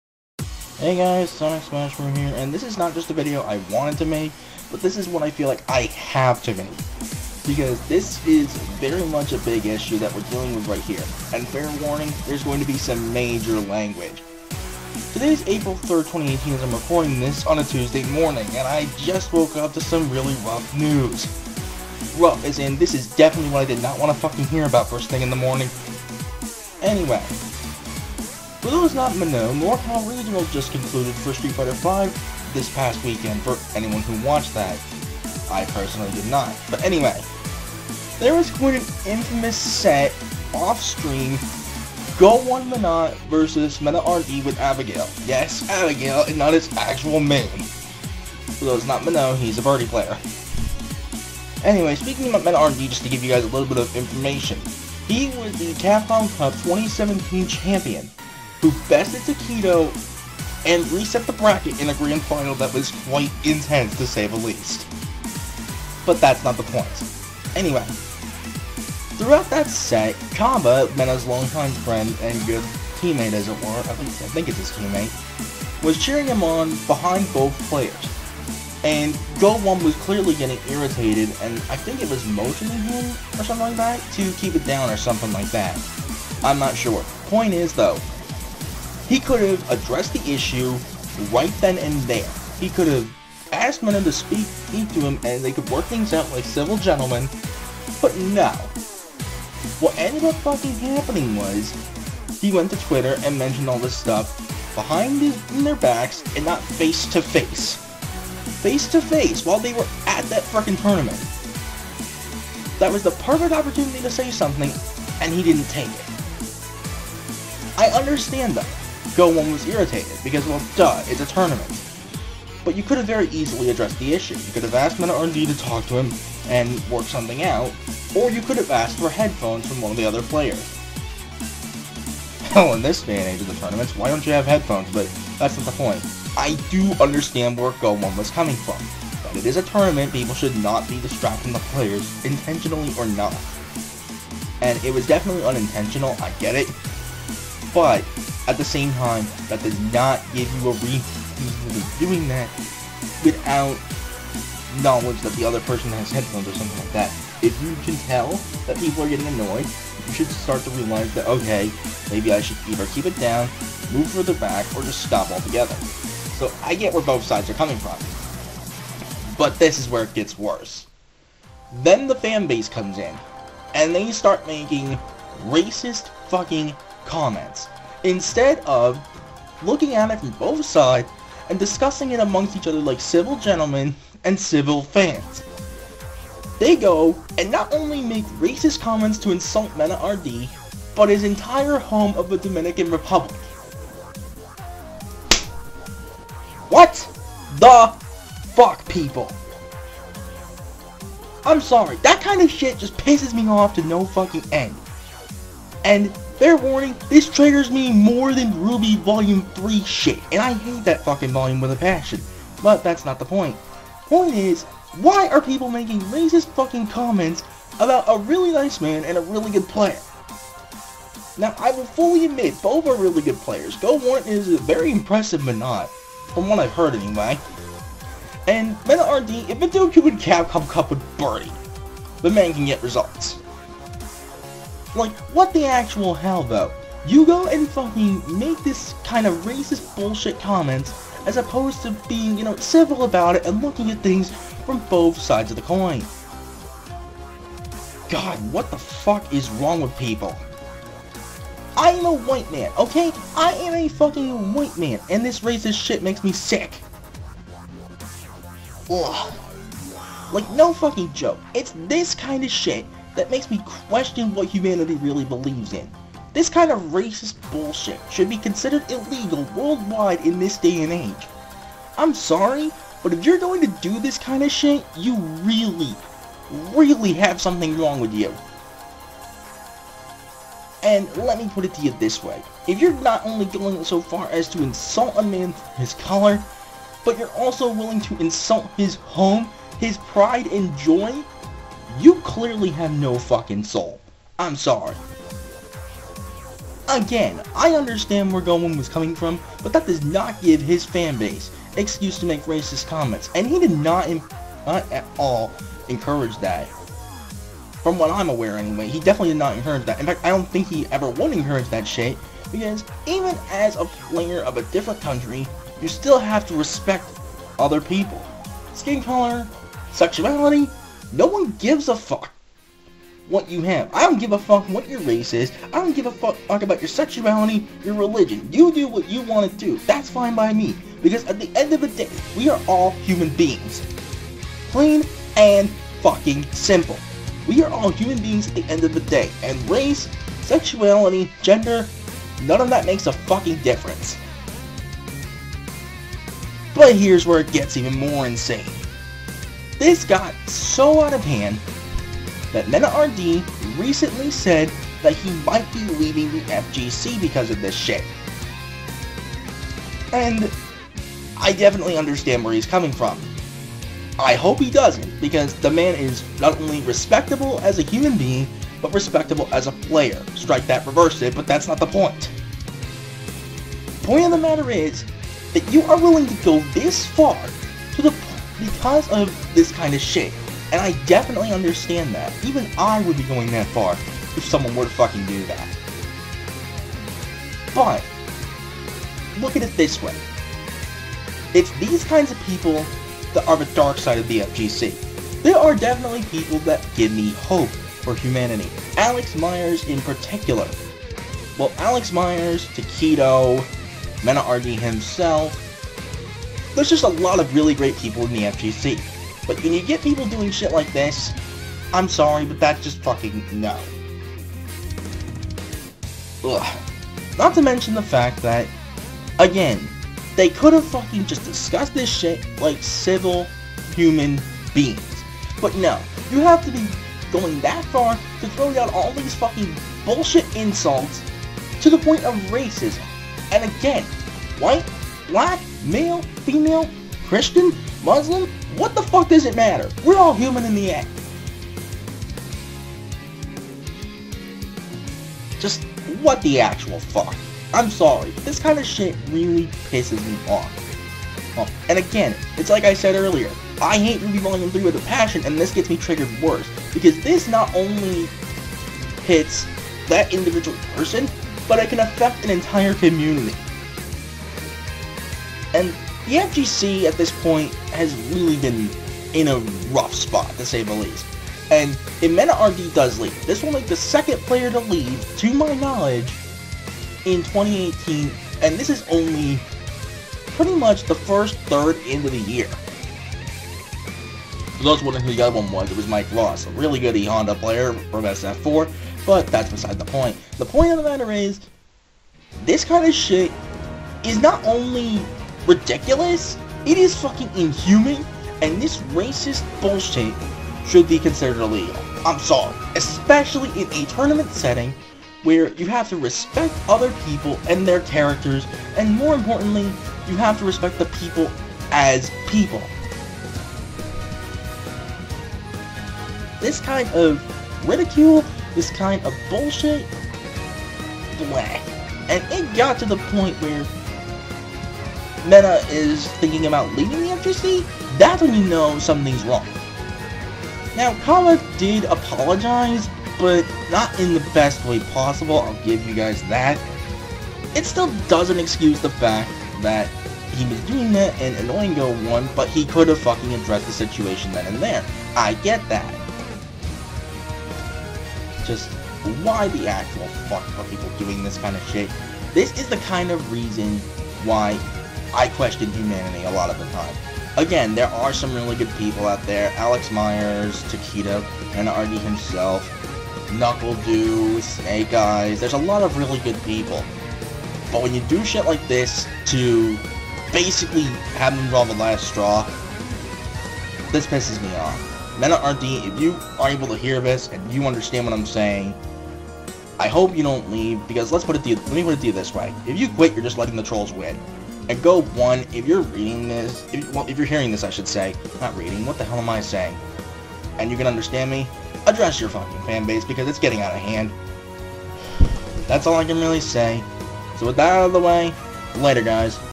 SFV Rant: GO1 vs. MenaRD Aftermath (Loud!)
I'm not keeping up with the CPT but this still has to be said...